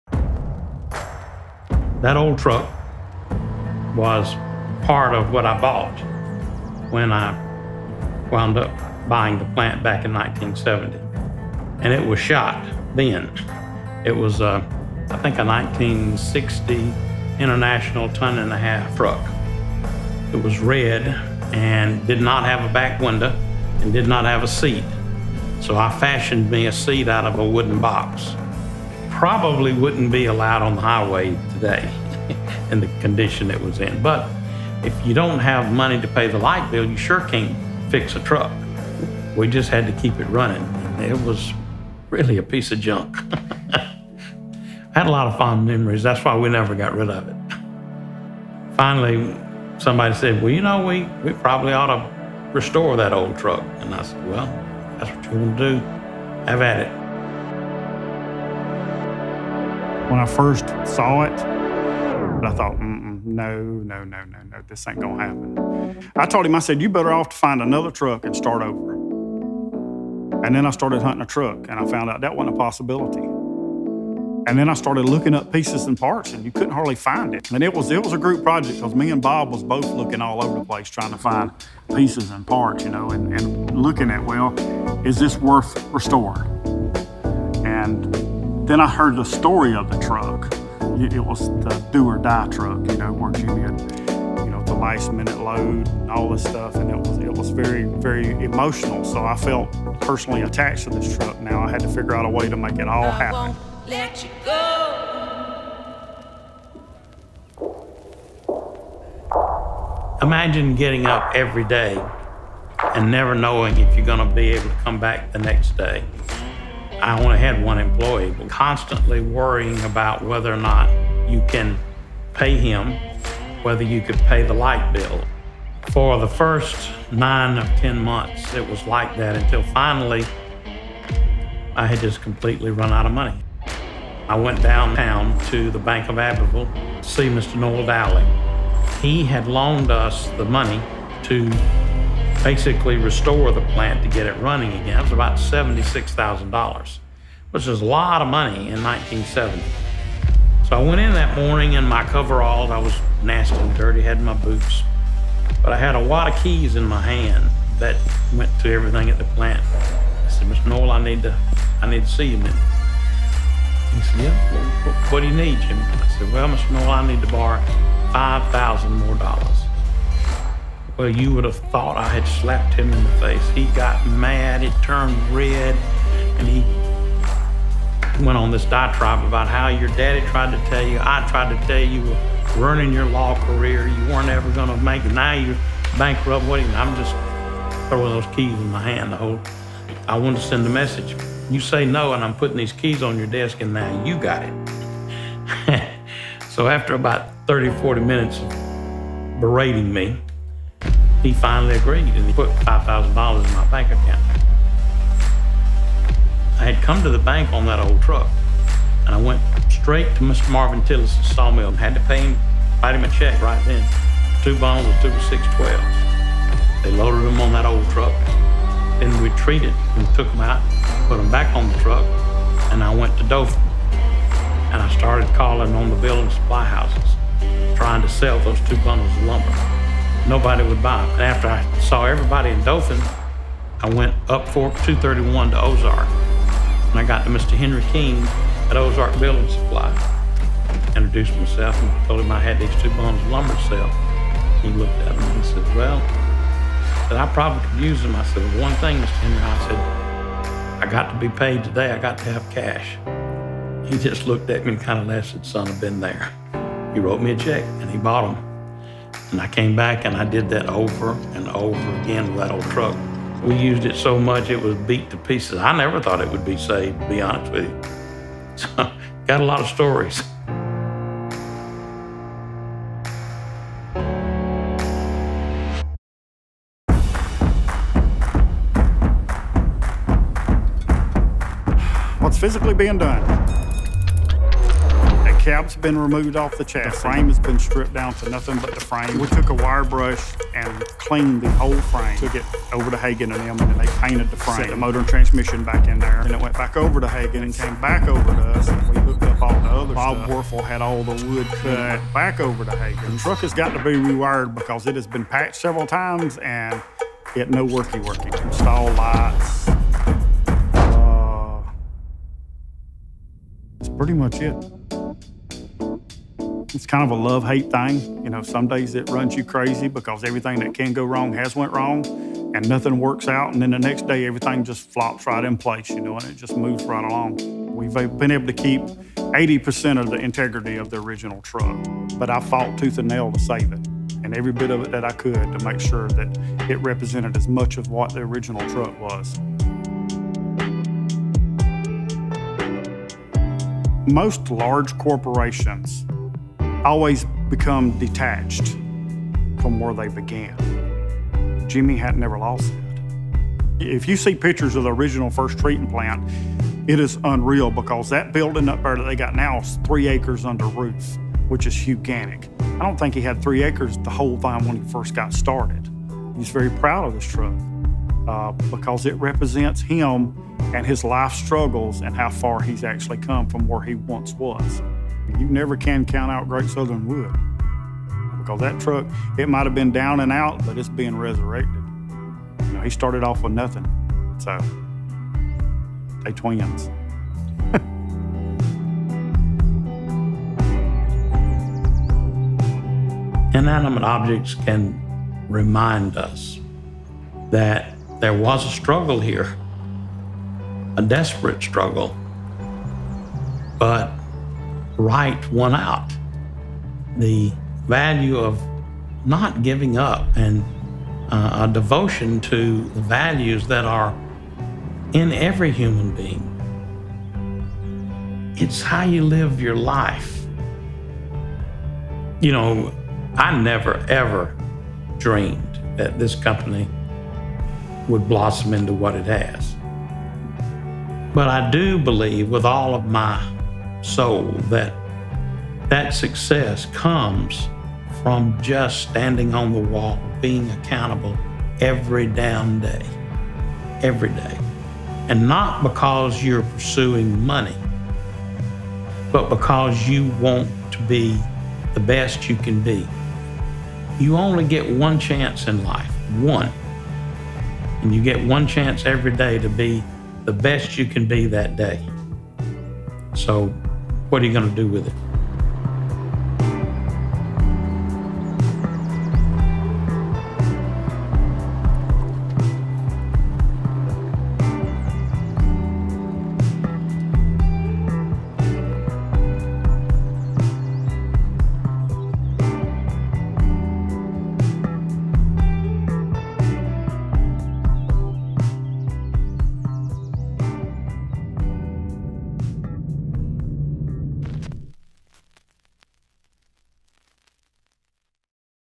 YellaWood | The Truck That Started It All
Jimmy Rane's first truck is more than just a beat-up old car—it represents the beginning of Great Southern Wood's journey and the determination of a young man whose hard work made something great.

Follow along for the story of GSW's humble beginnings and the resurrection of the vehicle that started it all.